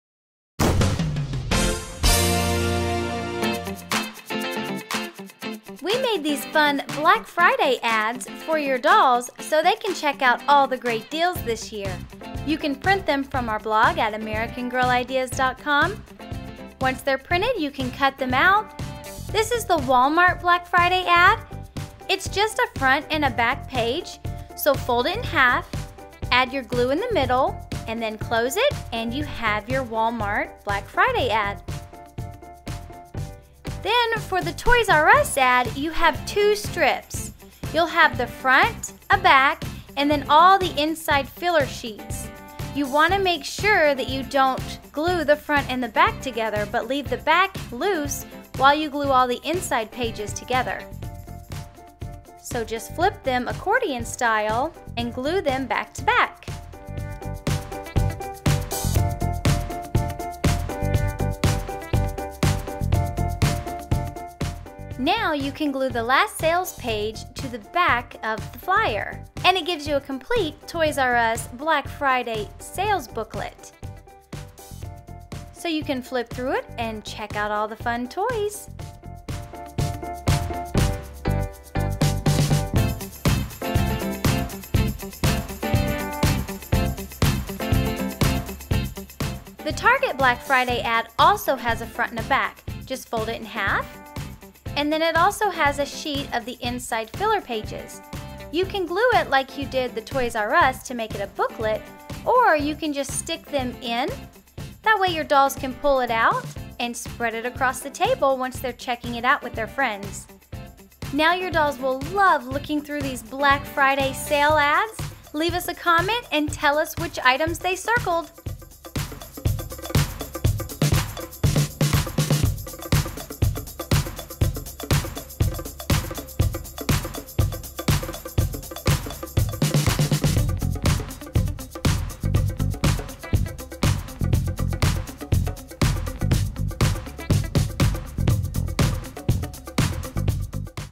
DIY Black Friday Ads
Learn how to make American Girl Doll Black Friday Ads. These doll crafts are fun and easy to make. You can print these Black Friday Doll Ads for your dolls to look at too.